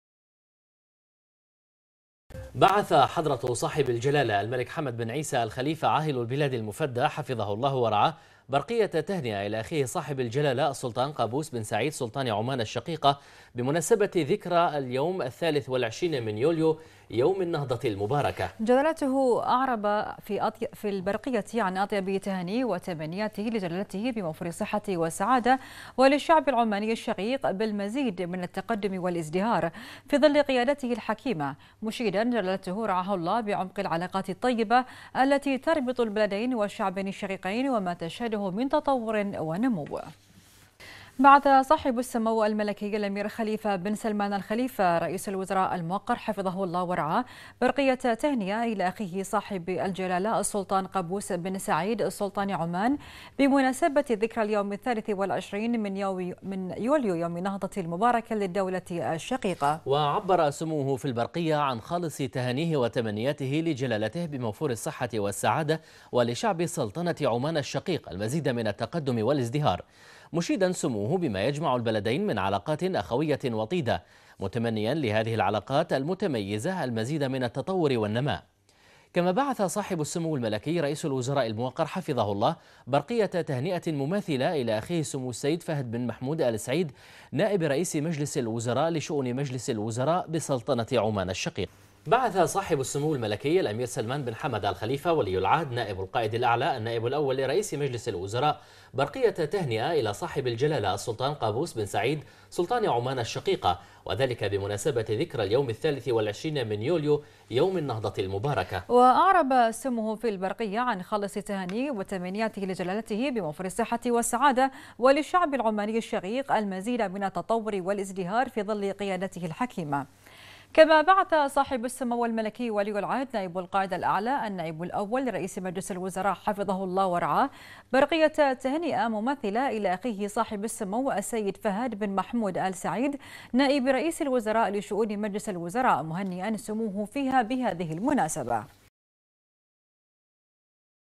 البحرين: القيادة الحكيمة تهنئ سلطان سلطنة عمان بمناسبة ذكرى يوم النهضة
المنامة في 23 يوليو / بنا / بعث حضرة صاحب الجلالة الملك حمد بن عيسى آل خليفة عاهل البلاد المفدى حفظه الله ورعاه برقية تهنئة إلى أخيه صاحب الجلالة السلطان قابوس بن سعيد سلطان عمان الشقيقة وذلك بمناسبة ذكرى اليوم الثالث والعشرين من يوليو يوم النهضة المباركة.